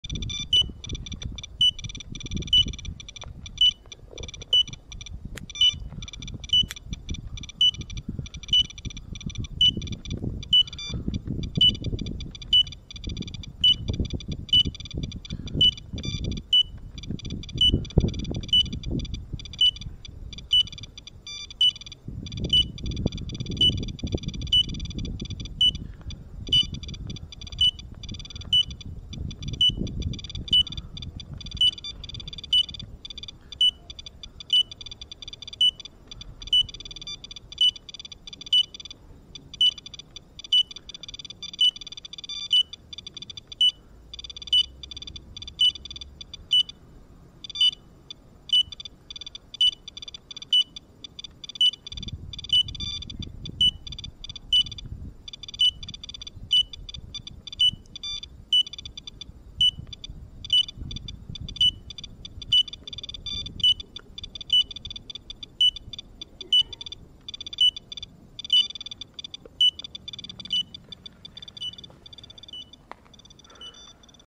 Aug 20, 2011 radiation check with SOEKS and the DP802i geiger counters on the outskirts of Tokyo
Well it has been over 5 months since the "event" in Fukushima, and as you can see the radiation is not going anywhere anytime soon.  This reading is from the side of the street where there is good water run-off.  I guess all the hard rain we have had here hasn't really helped wash away much of the fallout.  Keep in mind this is on the outskirts of Tokyo, in Kashiwa.